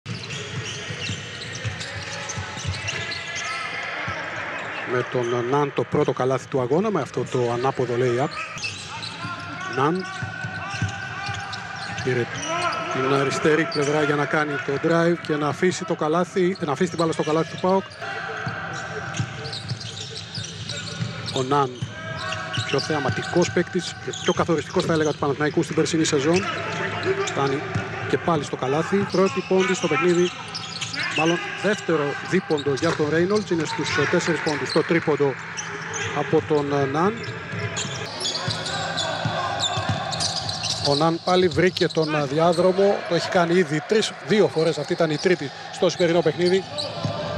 ΠΑΟΚ - Παναθηναϊκός | Ασταμάτητος στην 1η περίοδο ο Ναν | 12/10/2024 | ΕΡΤ
Greek Basketball League (GBL) | 2η Αγωνιστική, ΠΑΟΚ - Παναθηναϊκός | Ο άσος του Παναθηναϊκού πέτυχε 11 πόντους στην 1η περίοδο.